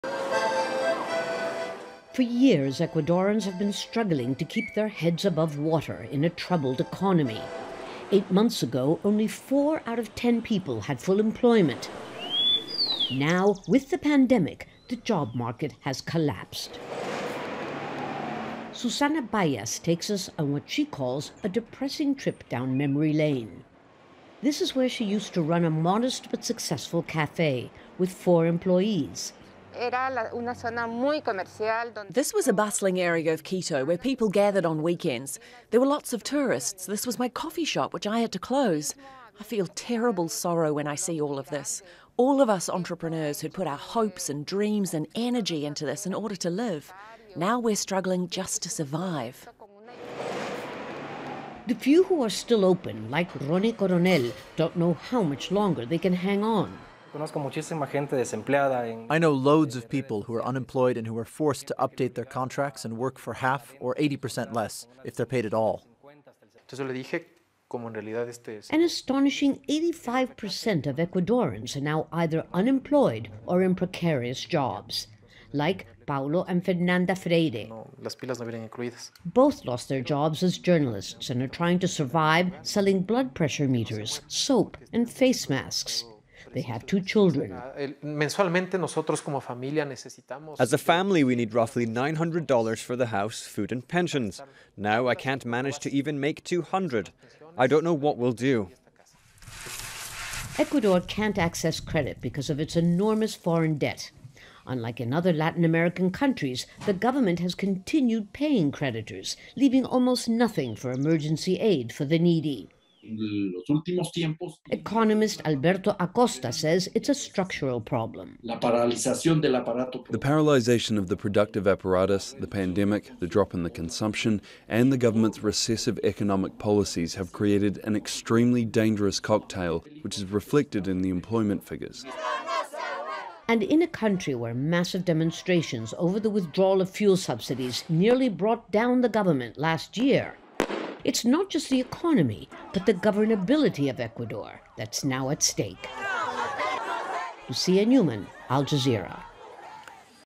Ecuador faces worst unemployment crisis in Latin America
Early on in the pandemic, Ecuador took the lead with the highest number of COVID-19 related deaths in Latin America. Now, it is leading the region in unemployment. A shocking 85 percent of Ecuadorans are now either unemployed or in precarious jobs.

Al Jazeera's Lucia Newman reports.